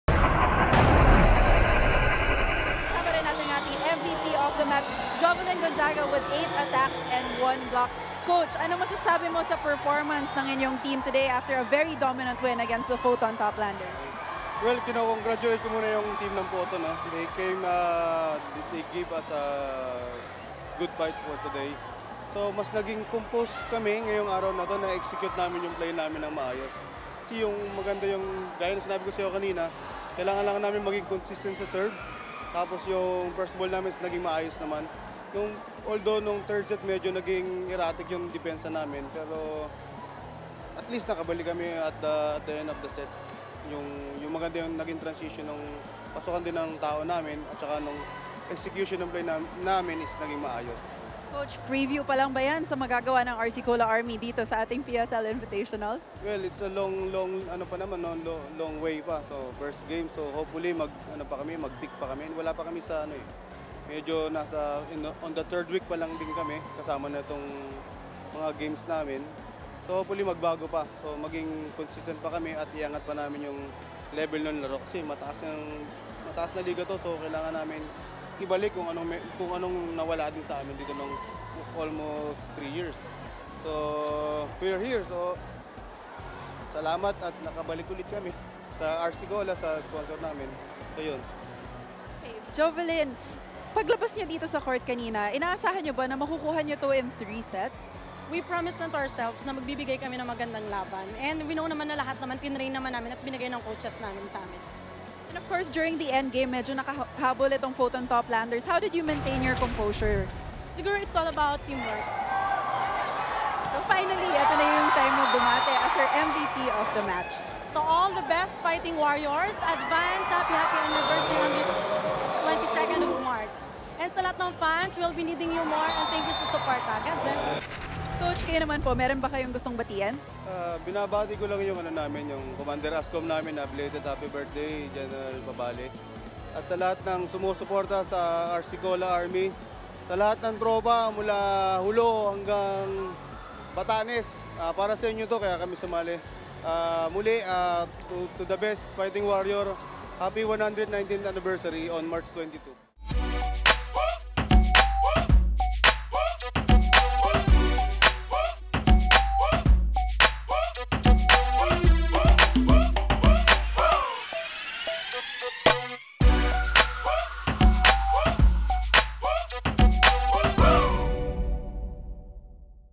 Match MVP: Jovelyn Gonzaga | 2016 PSL Invitational
Jovelyn Gonzaga was named Best Player of the RC Cola vs. Foton game.